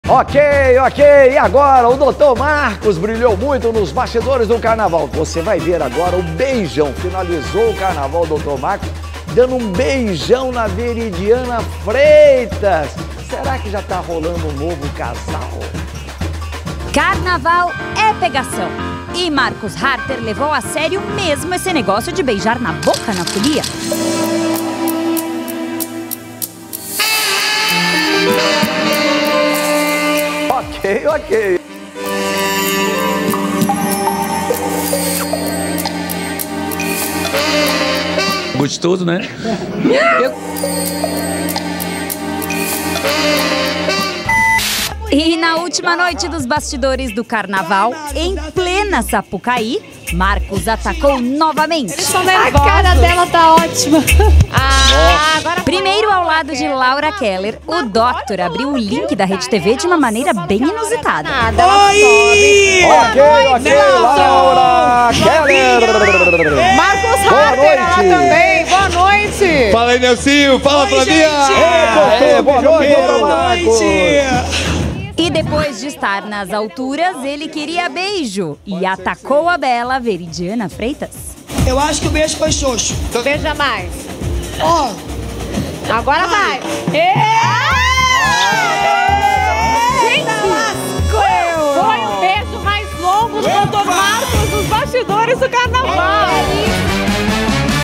Confira os melhores momentos de Marcos Harter no Carnaval
O médico Marcos Harter não perdoou ninguém e beijou todas no carnaval. Na lista estão Veridiana Freitas, Geisy Arruda, Denize Taccto e a musa da Gaviões.